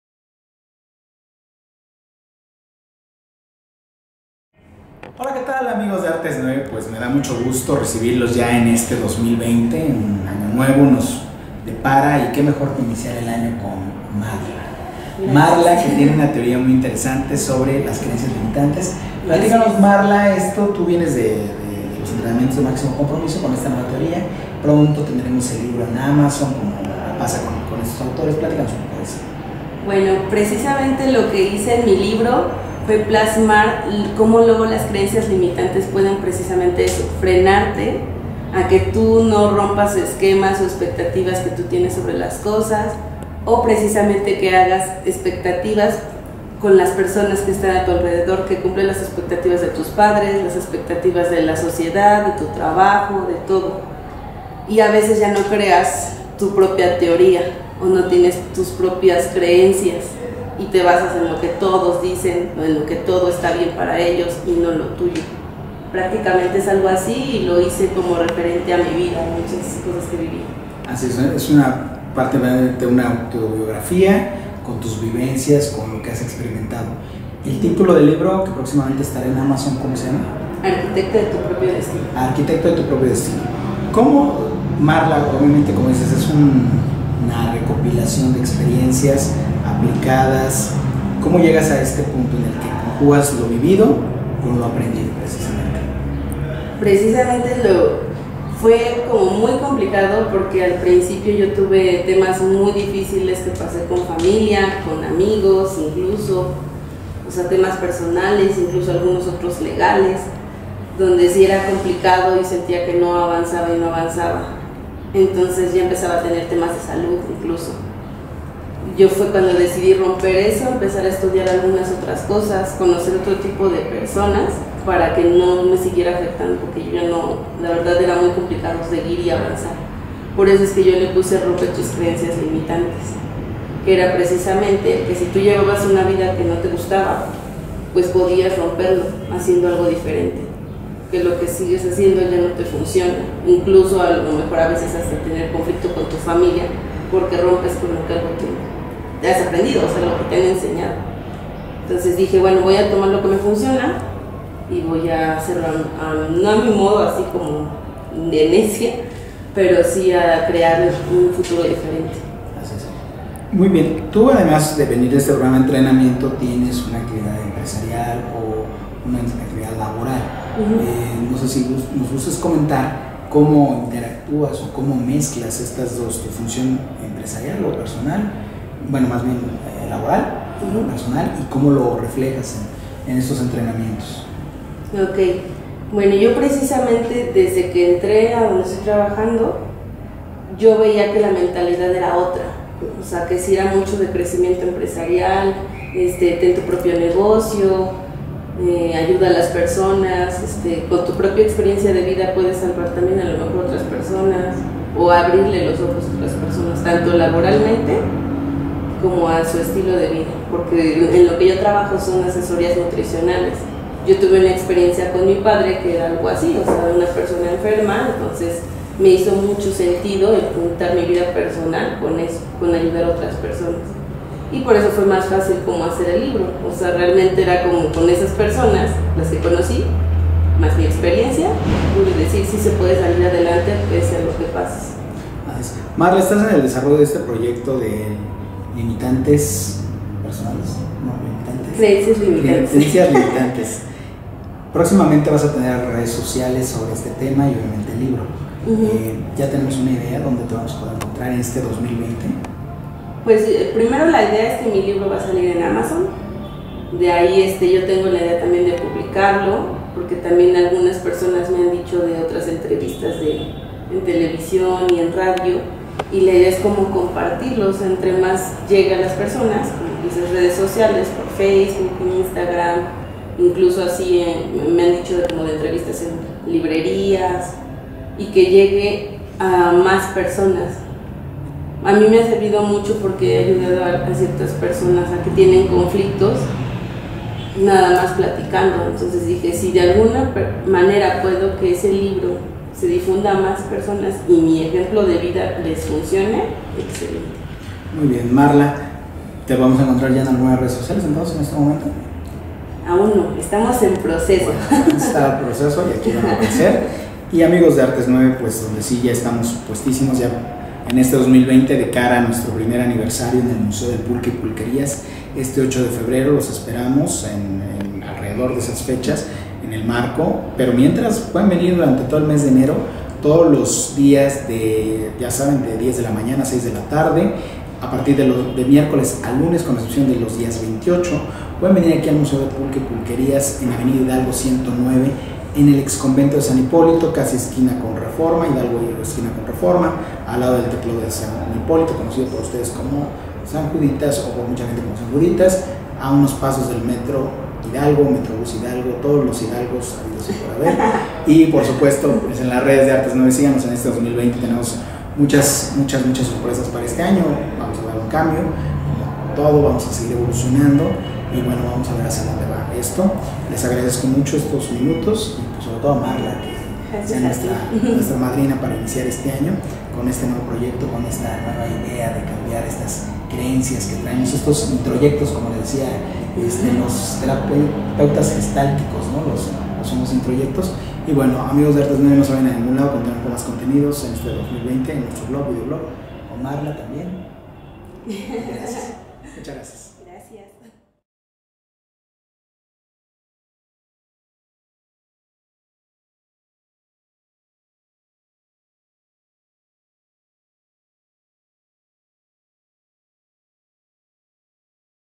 Marla en el Museo del Pulque y Pulquerias CDMX
Entrevista Exclusiva para Artes9.com presentamos a esta nueva escritora, que además es bailarina profesional, y de la que escucharemos muchos en 2020.